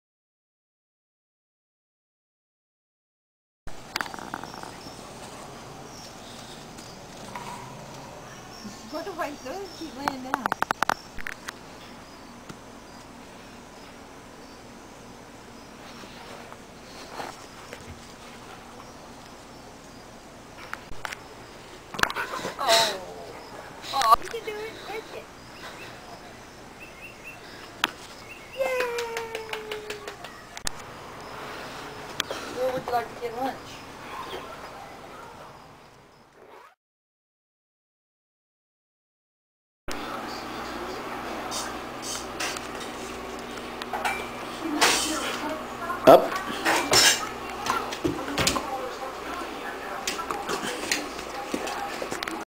rusty and the ball
Rusty will sometimes fetch the ball.
But most of the time.  He'll just stare at it.